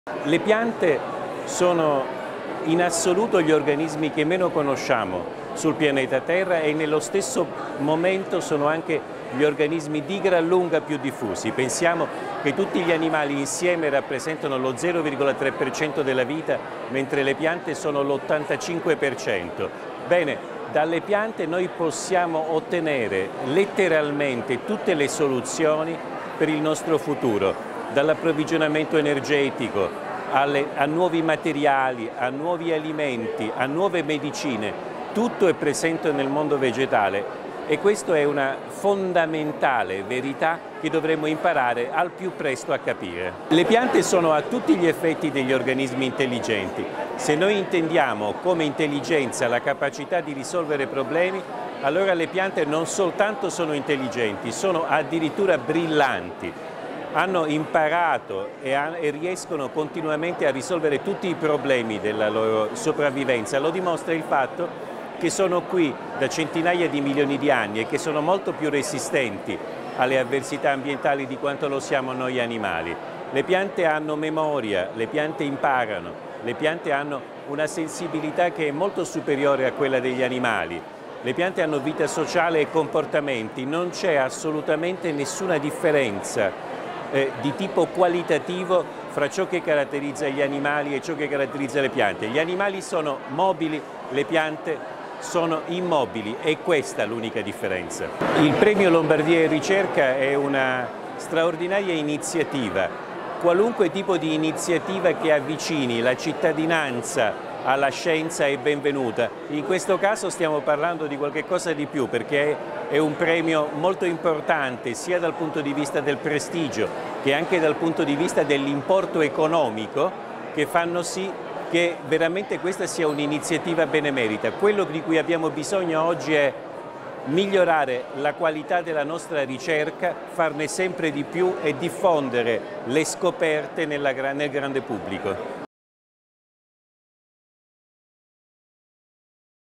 Stefano Mancuso: “Svelo la vita segreta delle piante, intelligenti e sociali”
L’inventore della neurobiologia vegetale sul palco della Giornata della Ricerca di Regione Lombardia